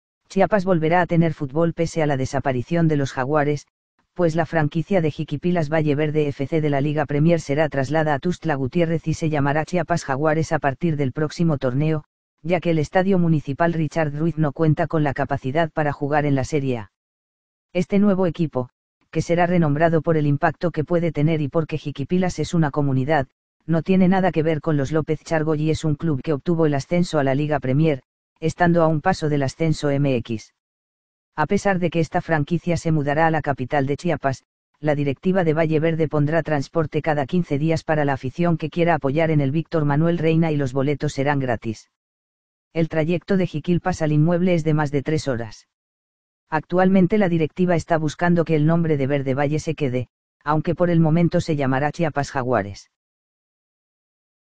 Jaguares de Chiapas resurgirá en el futbol mexicano!
Chiapas volverá a tener futbol pese a la desaparición de los Jaguares, pues la franquicia de Jiquipilas Valle Verde F.C. de la Liga Premier será traslada a Tuxtla Gutiérrez y se llamará Chiapas Jaguares a partir del próximo torneo, ya que el Estadio Municipal Richard Ruiz no cuenta con la capacidad para jugar en la Serie A.

Este nuevo equipo, que será renombrado por el impacto que puede tener y porque Jiquipilas es una comunidad, no tiene nada que ver con los López Chargoy y es un club que obtuvo el ascenso a la Liga Premier, estando a un paso del Ascenso MX.

A pesar de que esta franquicia se mudará a la capital de Chiapas, la directiva de Valle Verde pondrá transporte cada 15 días para la afición que quiera apoyar en el Víctor Manuel Reyna y los boletos serán gratis. El trayecto de Jiquilpas al inmueble es de más de tres horas.

Actualmente la directiva está buscando que el nombre de Verde Valle se quede, aunque por el momento se llamará Chiapas Jaguares.